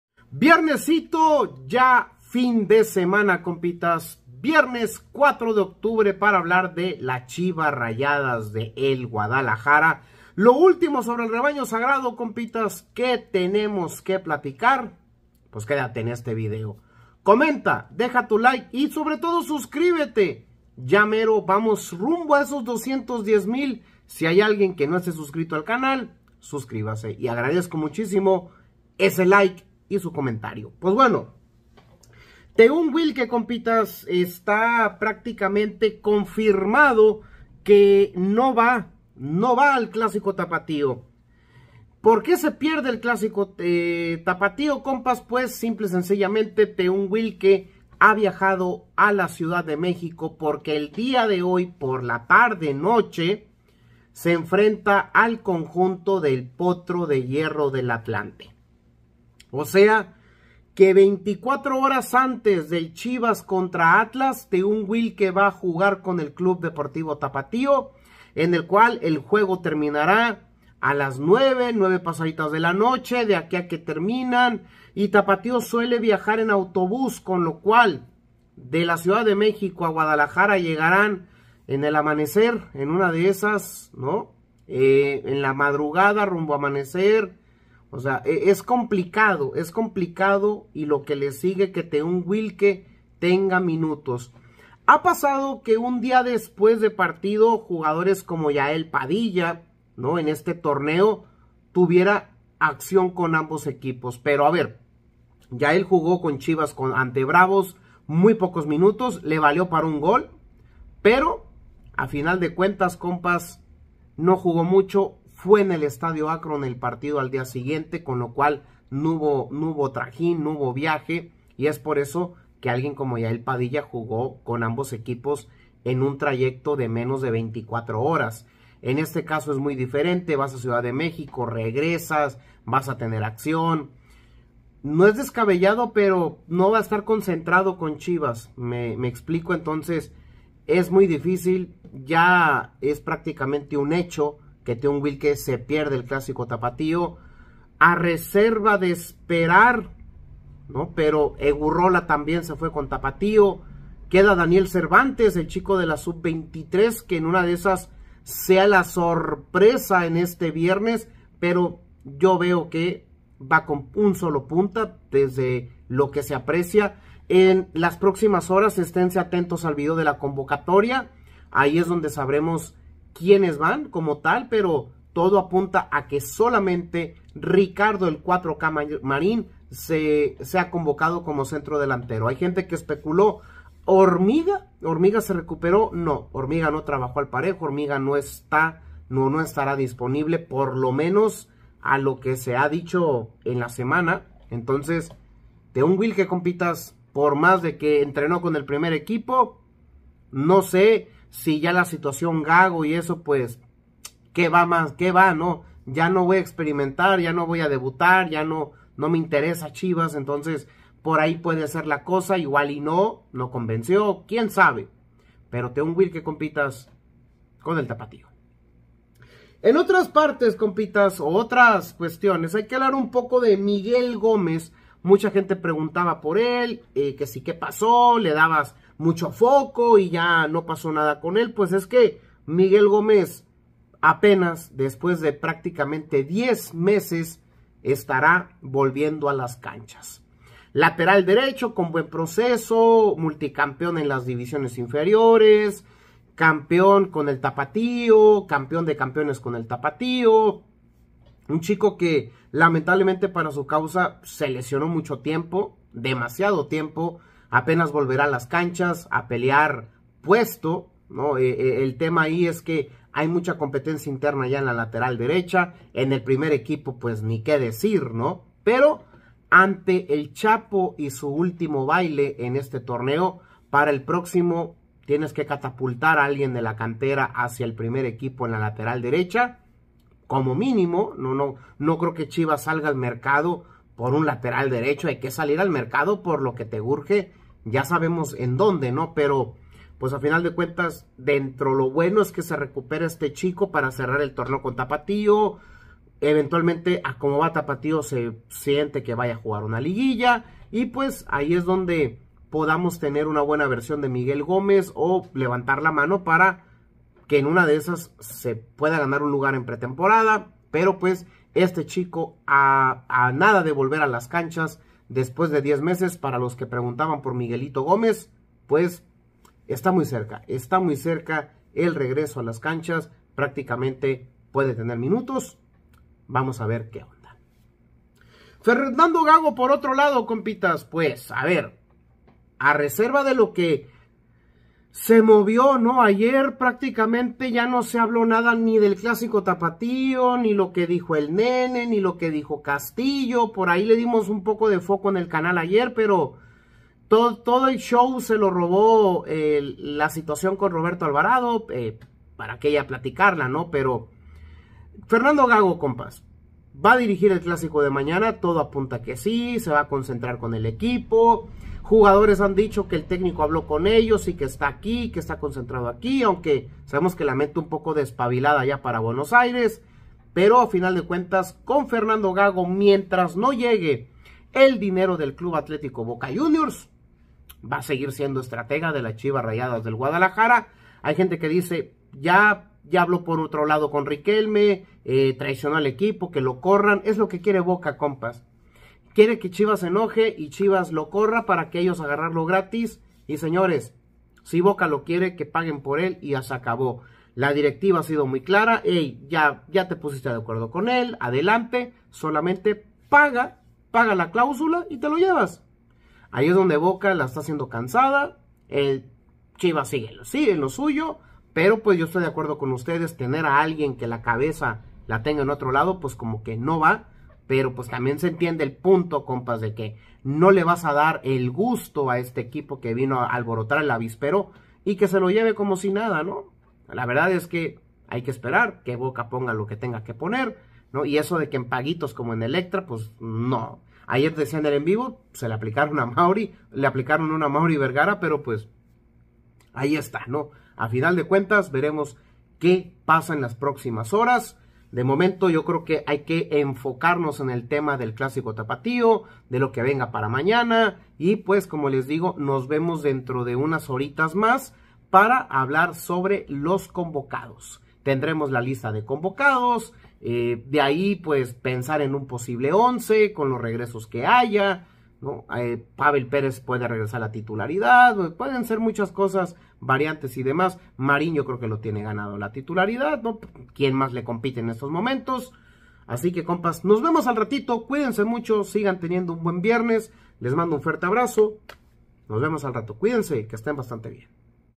🚨Teun Wilke SE PIERDE el clásico Tapatio, Jugador REGRESA después de 10 meses | Noticias Chivas Hoy
-TEUN WILKE SE PIERDE EL CLÁSICO TAPATÍO.

-MIGUEL GOMEZ REGRESA TRAS 10 MESES DE INACTIVIDAD POR LESIÓN

-QUÉ PASA CON FERNANDO GAGO?